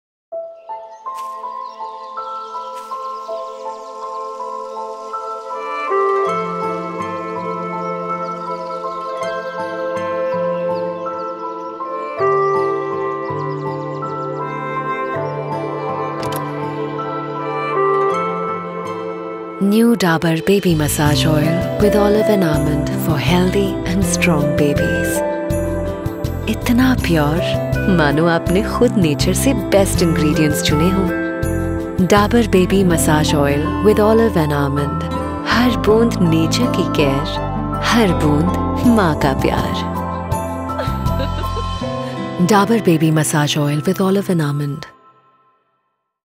Dabur Olive & Almonds
This Beautiful and effective Ad film of Dabur Olive & Almonds Directed by Nitish Sharma.